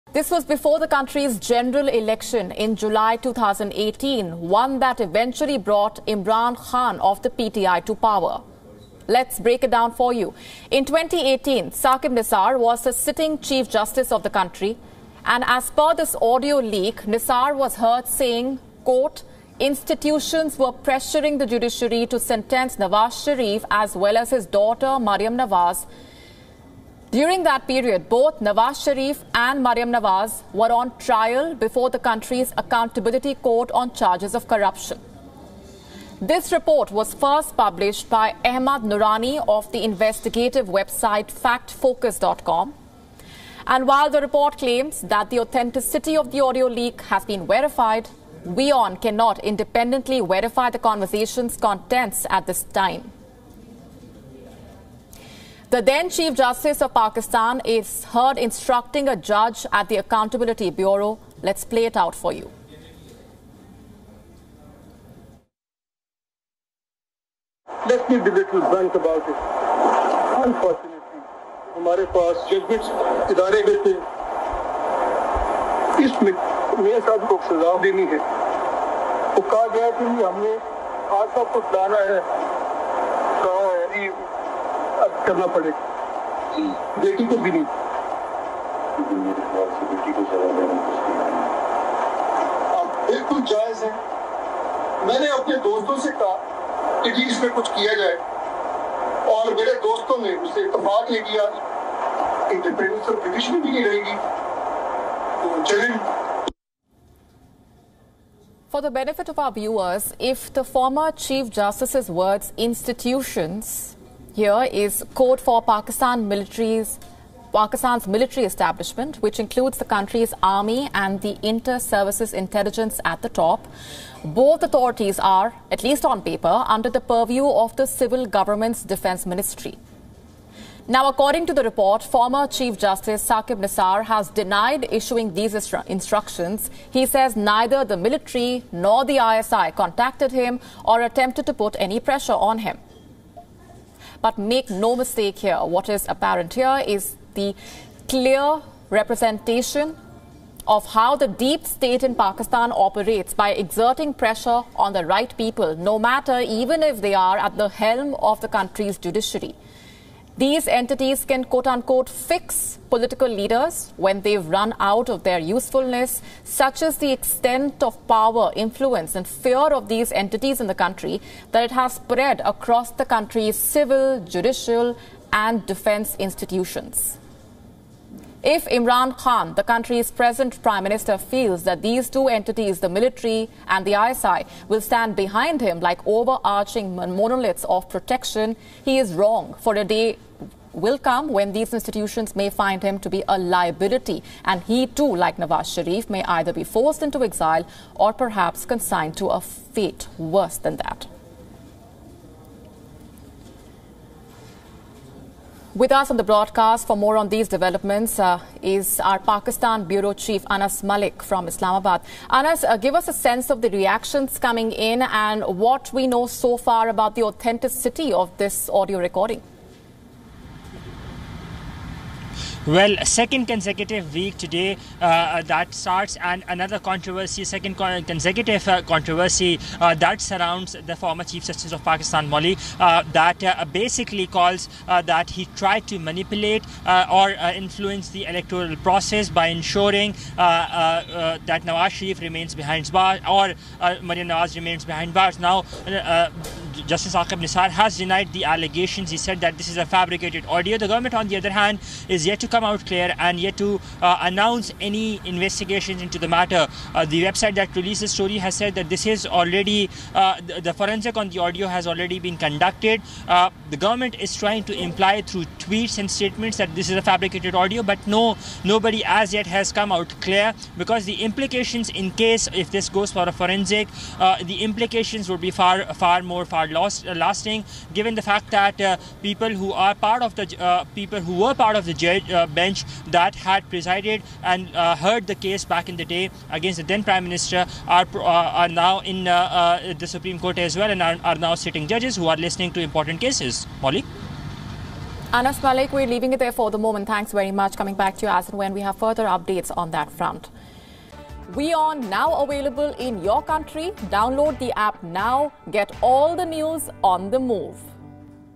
Leaked audio of ex-Pakistan CJ Saqib Nisar on Nawaz Sharif fabricated?
Former chief justice of Pakistan (CJP) Mian Saqib Nisar has termed an alleged leaked audio clip of his conversation about Nawaz Sharif, as “fabricated”.